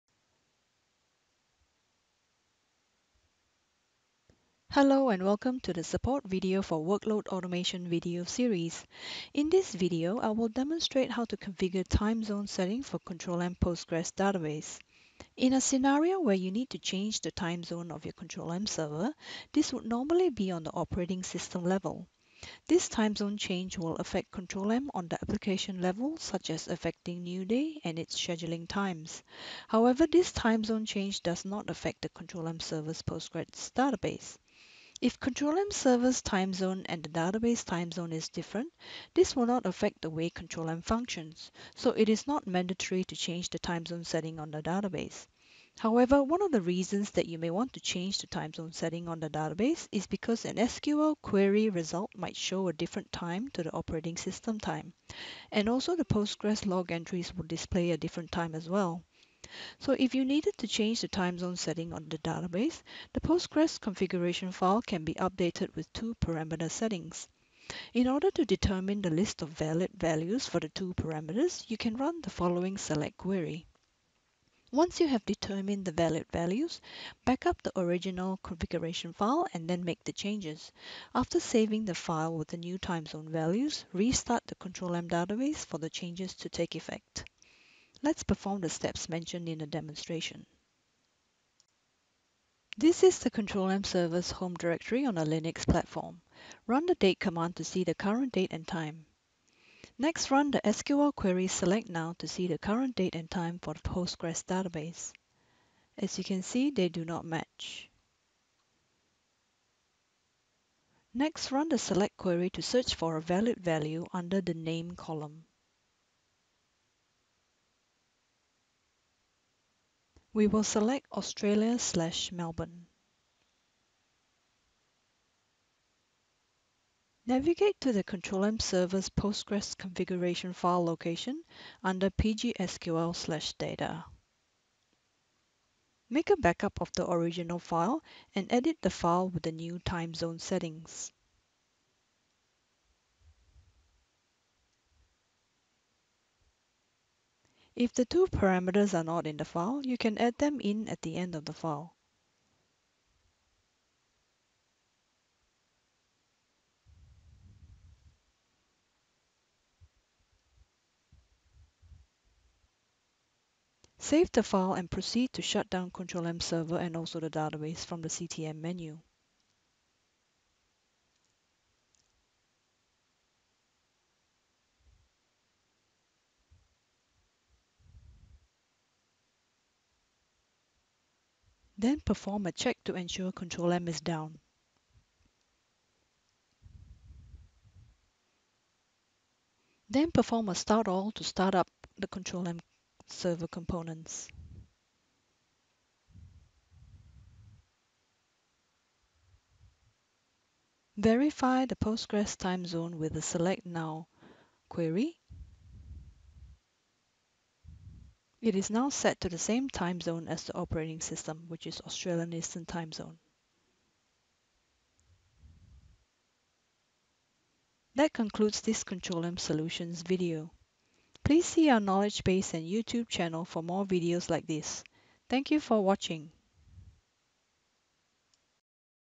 How to change time zone setting for PostgreSQL database
This video demonstrates how to change time zone setting for PostgreSQL database used by Control-M application.